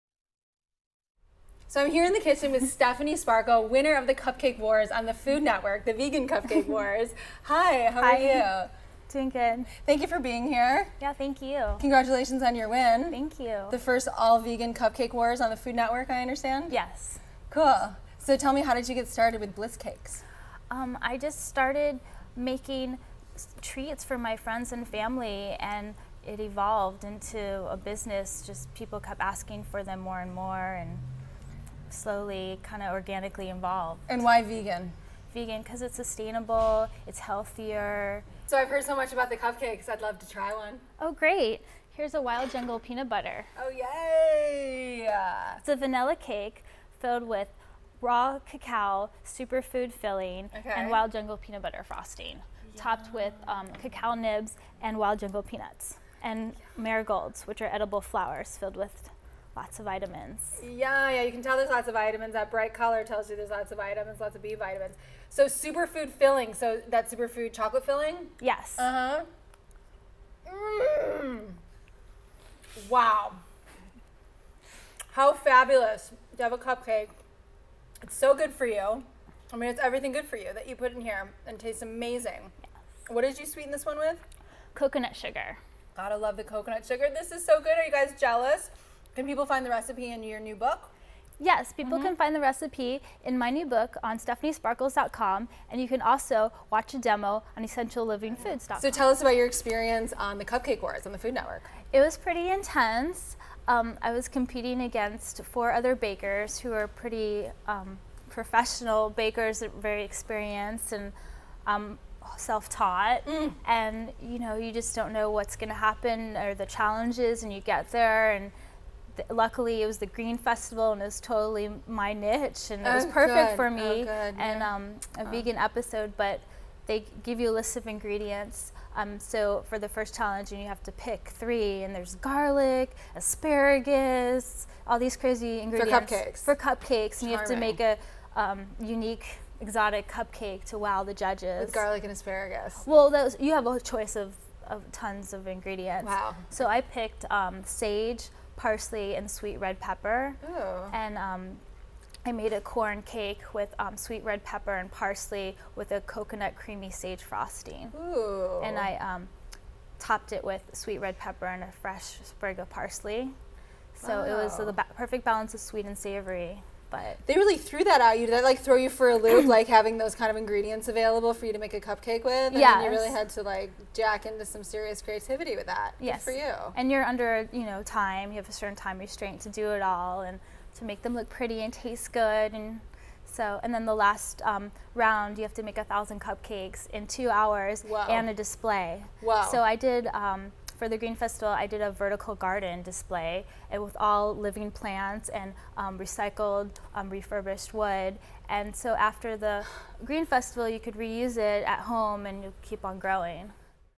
Cupcake Wars Winner Stephanie Sparkles Interview- Blythe Raw Live
Blythe interviews Cupcake Wars winner Stephanie Sparkles about her award winning cupcakes.

You can watch Blythe Raw Live on Fridays at noon pt